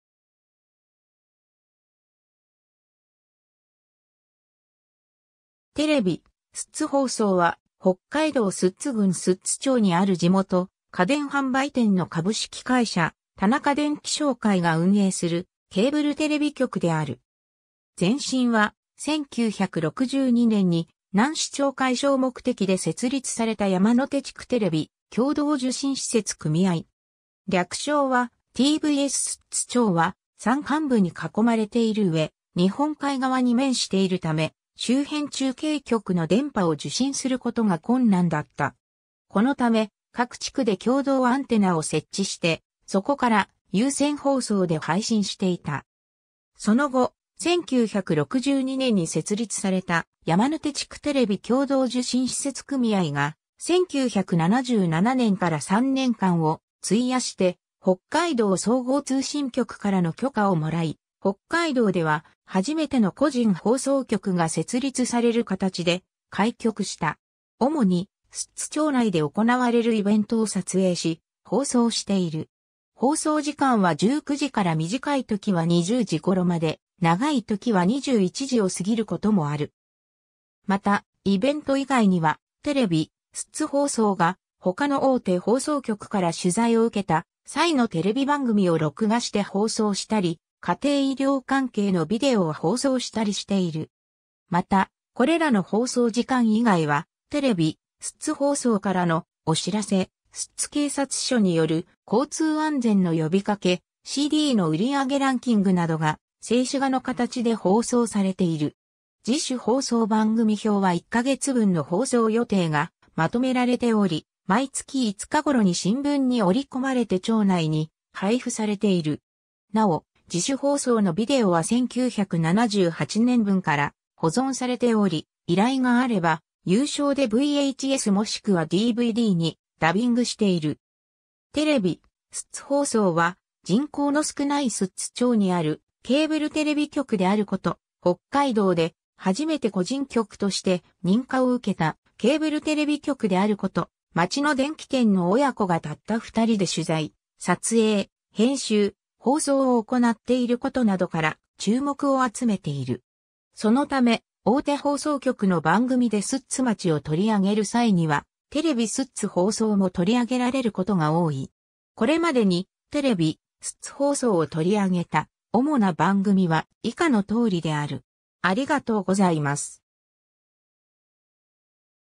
テレビ寿都放送
テレビ寿都放送（てれびすっつほうそう）は、北海道寿都郡寿都町にある地元家電販売店の株式会社田中電機商会が運営するケーブルテレビ局である。
前身は、1962年（昭和37年）に難視聴解消目的で設立された山の手地区テレビ共同受信施設組合。
略称はTVS 寿都町は山間部に囲まれている上、日本海側に面しているため、周辺中継局の電波を受信することが困難だった。
このため、各地区で共同アンテナを設置して、そこから有線放送で配信していた。
 その後、1962年に設立された山の手地区テレビ共同受信施設組合が、1977年から3年間を費やして北海道総合通信局からの許可をもらい、北海道では初めての個人放送局が設立される形で開局した。
 主に、寿都町内で行われるイベントを撮影し放送している。
放送時間は19時から短いときは20時頃まで、長いときは21時を過ぎることもある。
また、イベント以外には、テレビ寿都放送が他の大手放送局から取材を受けた際のテレビ番組を録画して放送したり、家庭医療関係のビデオを放送したりしている。
また、これらの放送時間以外は、テレビ寿都放送からのお知らせ、寿都警察署による交通安全の呼びかけ、CDの売り上げランキングなどが静止画のかたちで放送されている。
自主放送番組表は一ヶ月分の放送予定がまとめられており、毎月5日頃に新聞に折り込まれて町内に配布されている。
なお、自主放送のビデオは1978年分から保存されており、依頼があれば有償でVHSもしくはDVDにダビングしている。
 テレビ寿都放送は、人口の少ない寿都町にあるケーブルテレビ局であること、北海道で初めて個人局として認可を受けたケーブルテレビ局であること、町の電気店の親子がたった二人で取材・撮影・編集・放送を行っていることなどから、注目を集めている。
そのため、大手放送局の番組で寿都町を取り上げる際には、テレビ寿都放送も取り上げられることが多い。
これまでに、テレビ寿都放送を取り上げた主な番組は以下の通りである。